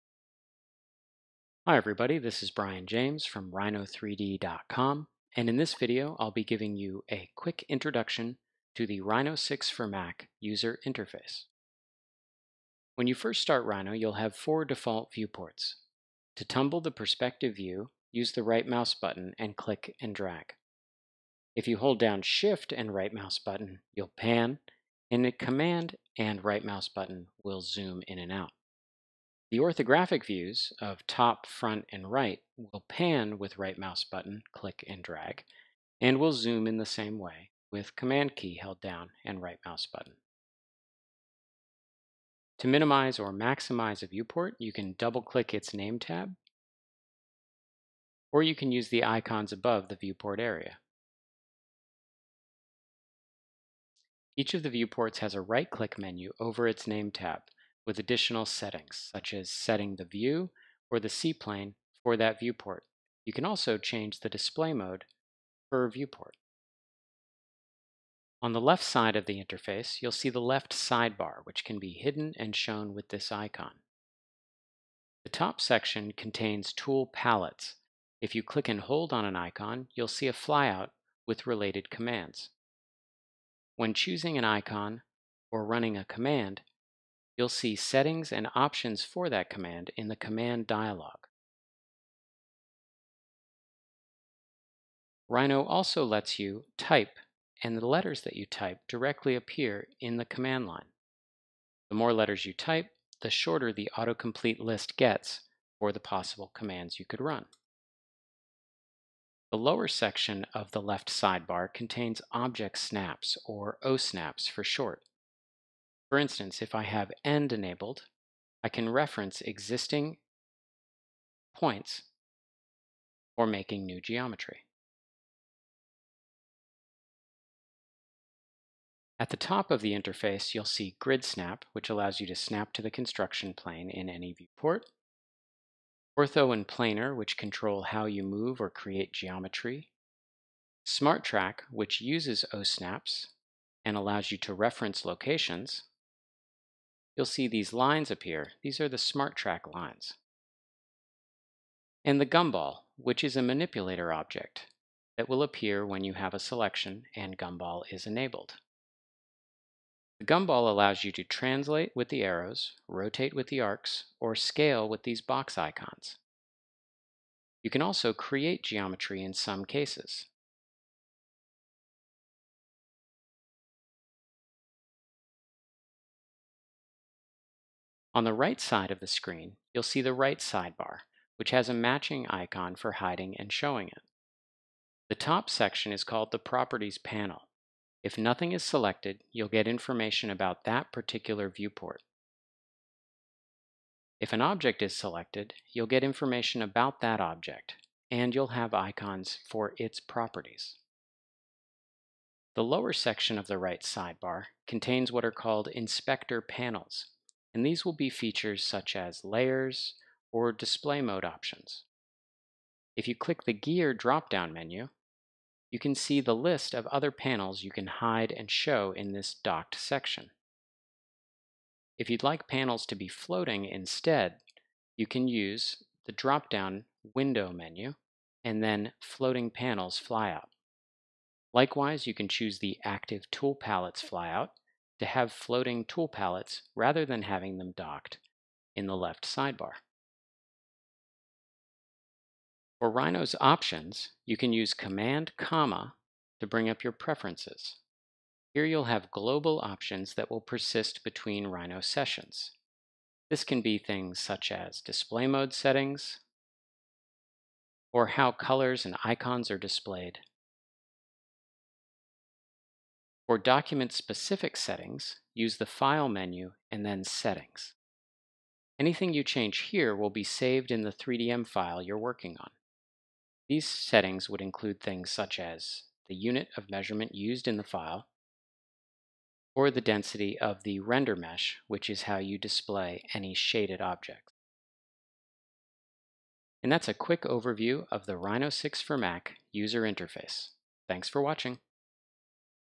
Intro to the Rhino 6 for MAC Interface
Watch Brian James, tech and trainee at McNeel, walk through the new features in the Rhino 6 for MAC UI. He'll show the different components of the interface, such as the inspector panels,  viewports & tool palettes. You'll learn to rotate, pan and zoom the views as well as a quick overview of modeling helpers such as the Gumball and Smarttrack.